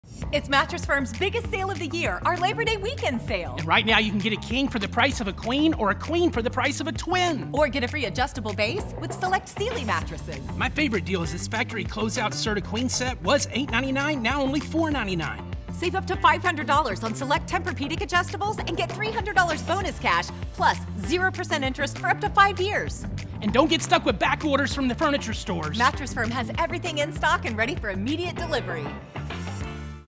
Mattress Firm Tulsa Labor Day Weekend Sale
Mattress Firms Biggest Sale Of The Year!
King for the price of a queen or queen for the price of a twin.
Free adjustable base with select Sealy mattresses.
Factory closeout Serta queen set was $899 now $499
Save up to $500 off select TempurPedic adjustable sets plus get $300 bonus cash.
0% interest financing up to 5 years.
In stock and ready to go.